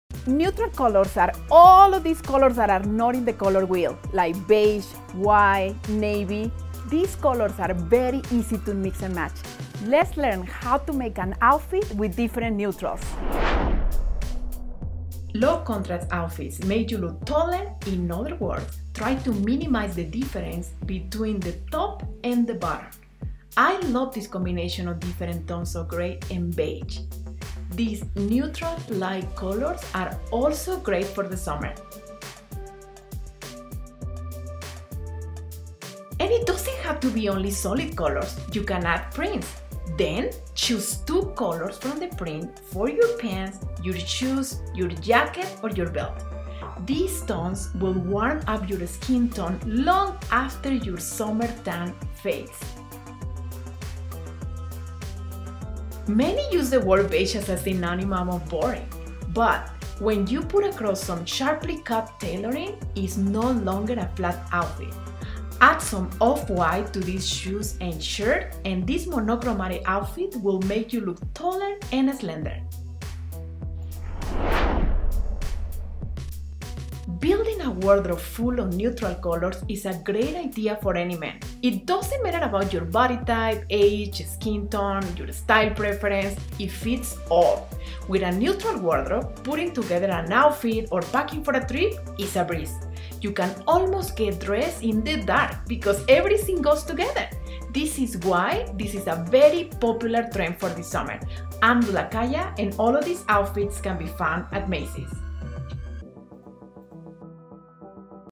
Neutral Wardrobe
Neutrals are becoming very popular for men this 2020. Let’s decode the rules for neutral dressing. Neutral Colors are any color that is not in the color wheel, like beige, gray, white. And all of them go with each other.

Building a wardrobe full of neutral colors is a great idea for any man – regardless of body type, age, skin tone or style preferences. With a neutral wardrobe, putting together an outfit or packing for a trip is a breeze. You can almost get dressed in the dark because everything goes together.
This is why this is a very popular trend this summer

Let’s learn different ways to wear all neutrals in one outfit💁🏼‍♂️